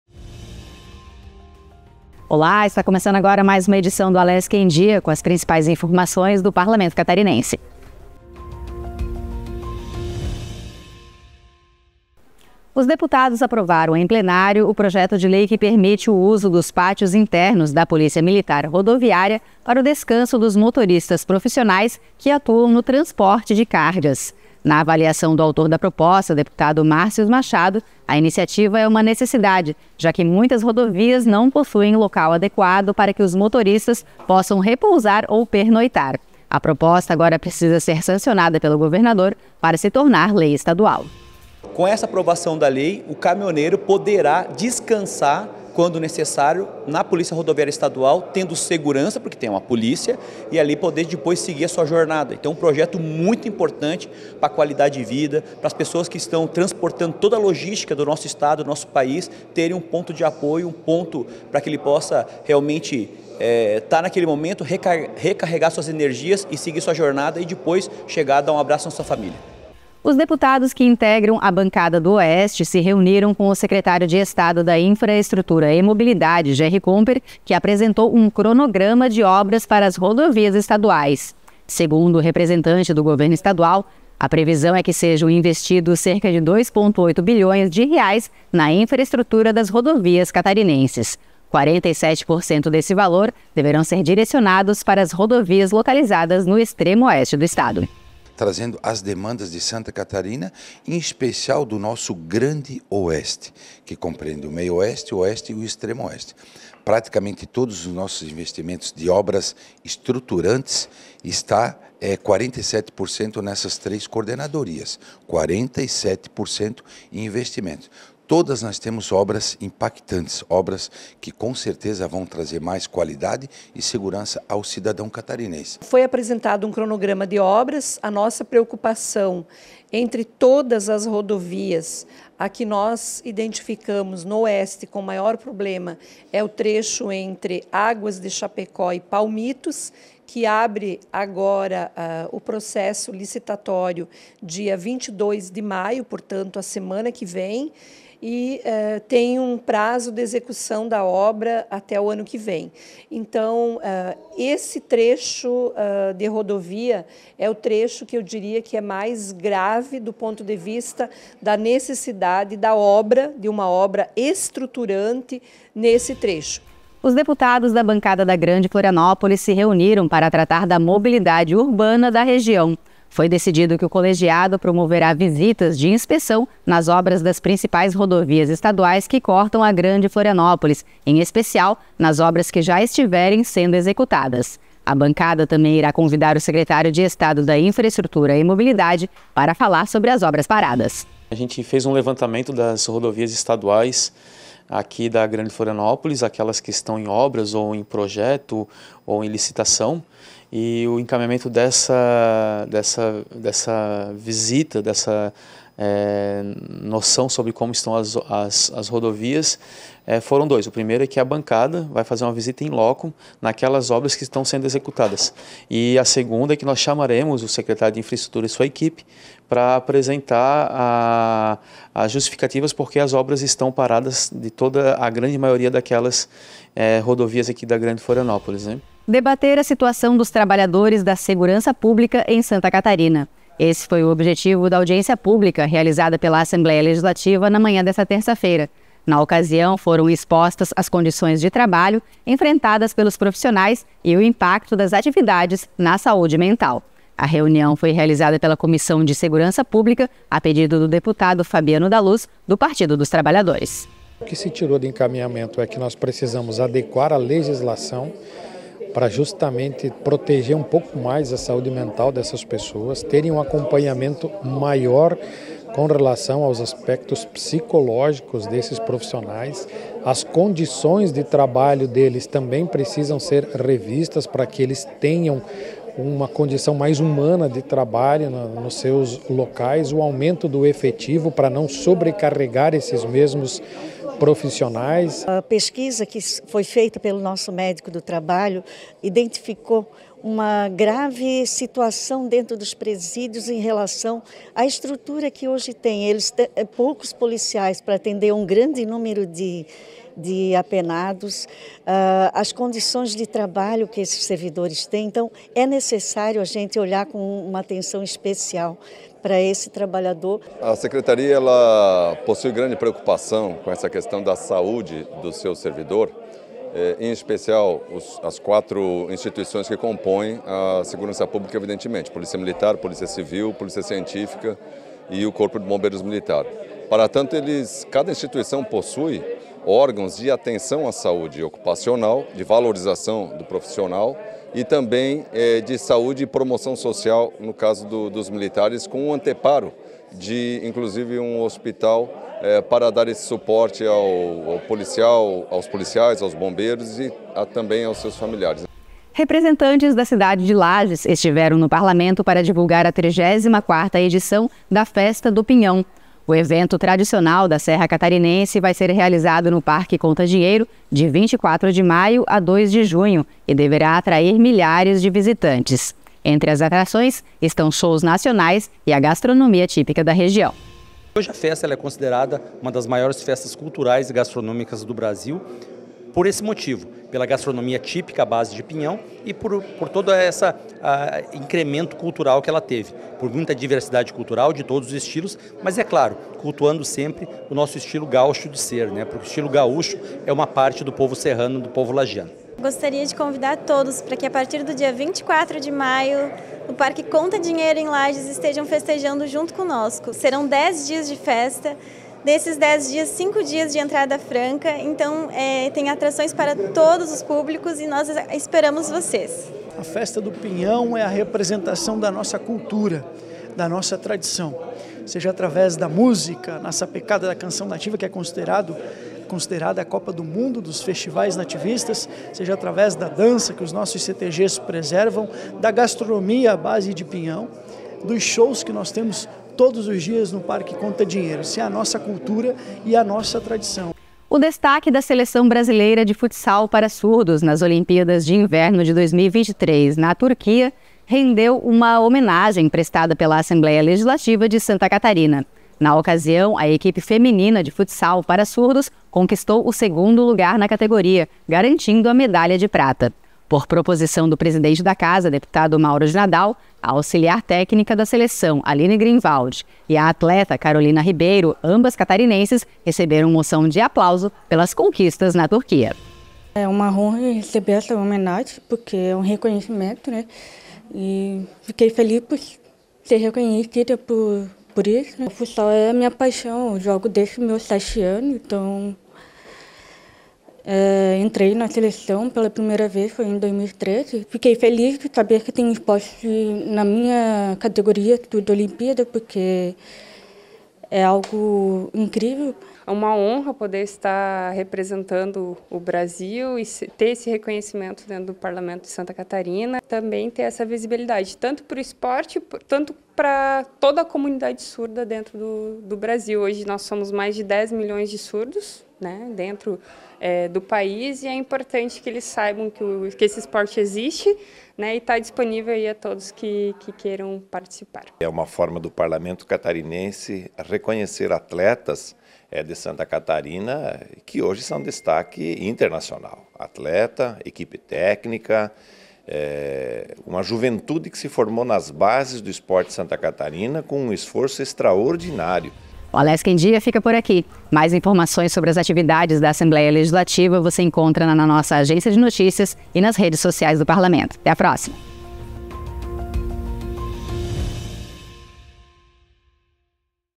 Alesc em Dia - Edição da Tarde - 14/05/24
Alesc Em Dia é um resumo dos últimos acontecimentos da Assembleia Legislativa de Santa Catarina.

Aprovado uso de pátios de Polícia Militar Rodoviária de SC para descanso de caminhoneiros

Bancada do Oeste acompanha cronograma de obras para as rodovias estaduais da região

Bancada da GranFpolis define obras prioritárias para região e evento dos 10 anos do Plamus

Audiência pública aborda condições de trabalho dos policiais penais

Festa Nacional do Pinhão acontece de 24 de maio a 2 de junho

Medalhistas do futsal para surdos recebem moção de aplauso